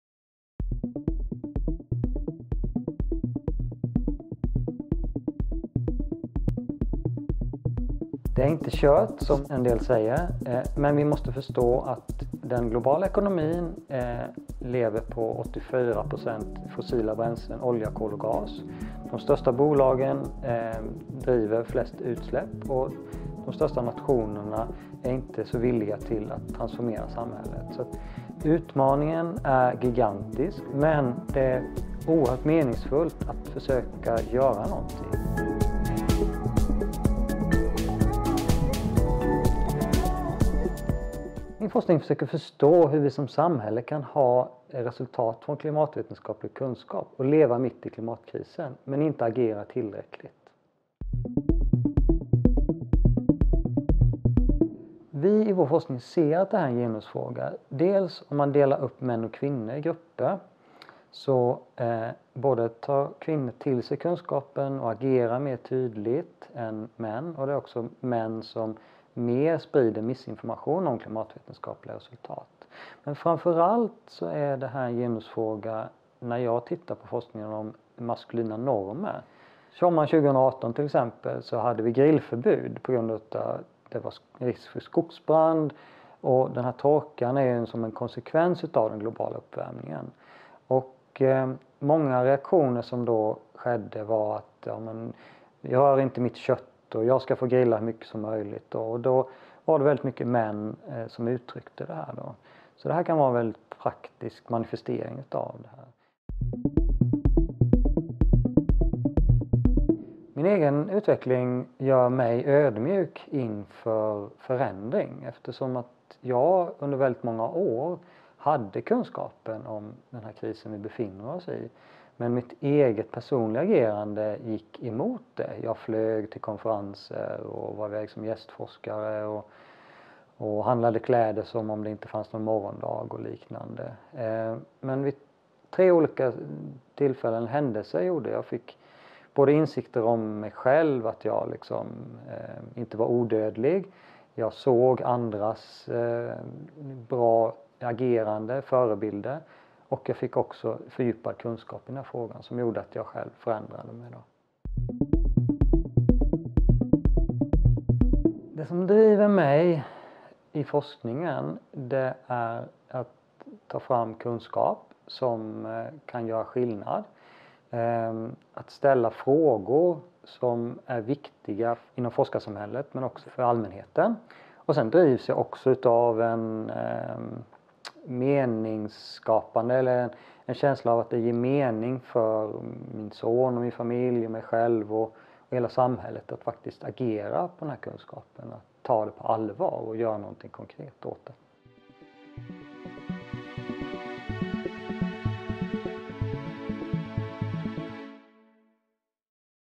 Alumni of the year 2021 - Martin Hultman
Martin Hultman, docent at Chalmers, is alumni of the year 2021.